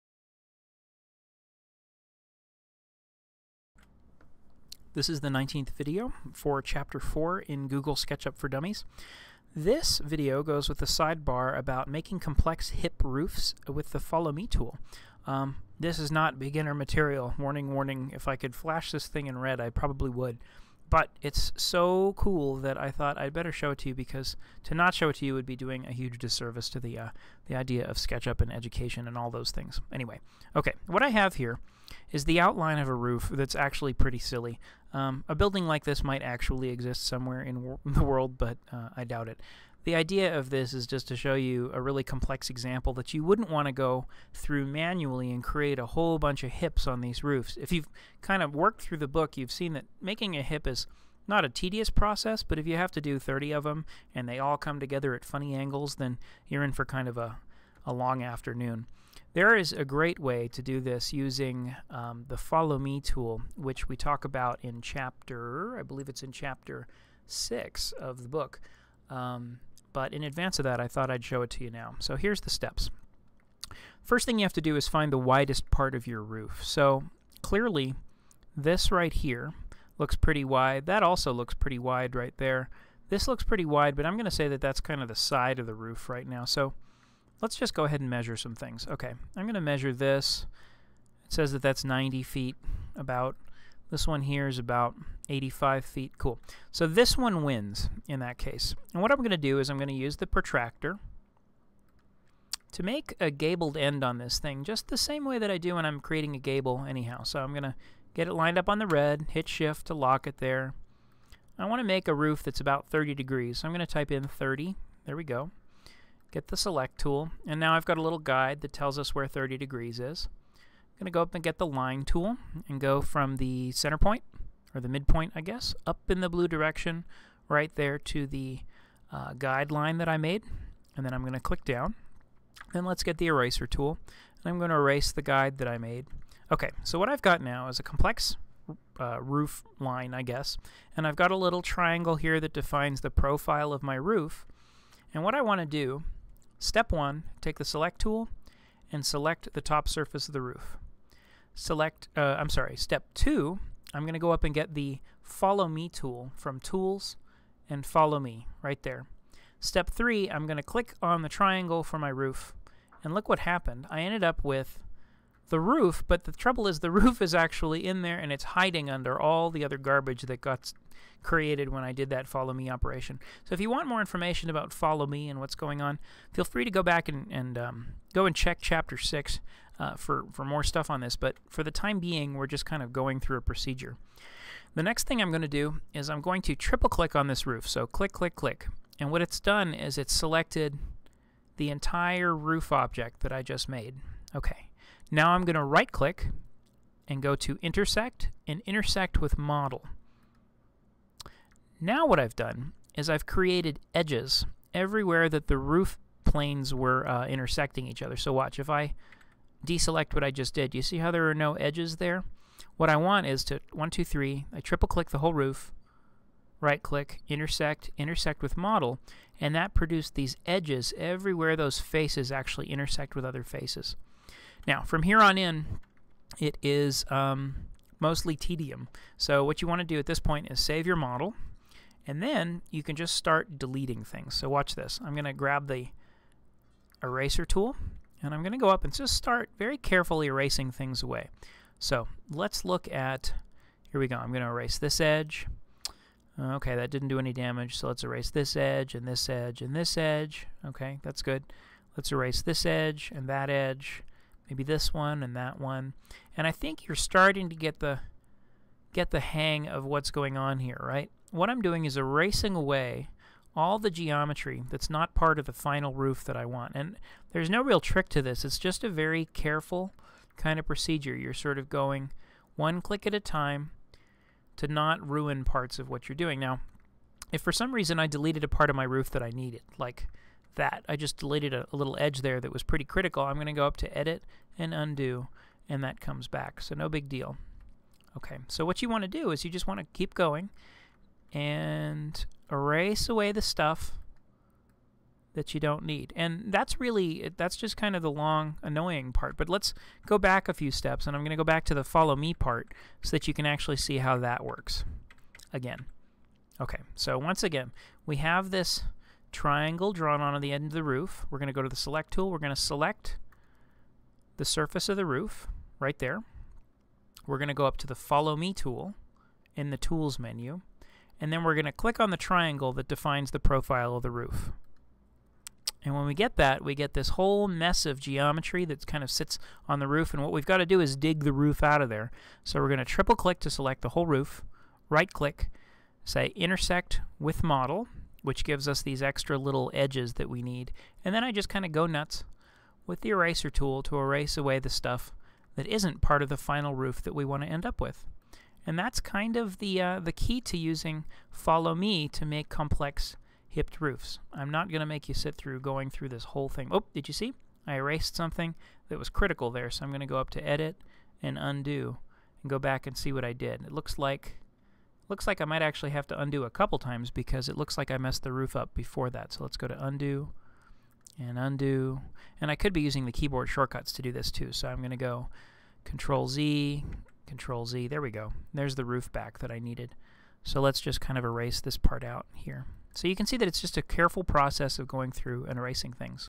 SketchUp: Complex hip roofs and the Follow Me tool
This video accompanies Google SketchUp for Dummies, by Aidan Chopra. For more information about the book, please